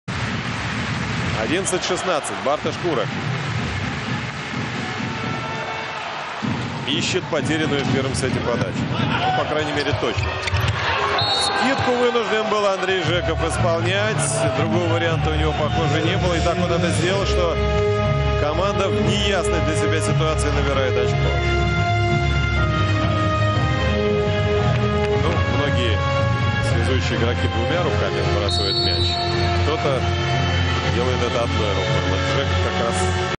Zhekov one hand tip
Zhekov shows master's ability during match with Poland in World League Final Eight, group A, Gdansk, Poland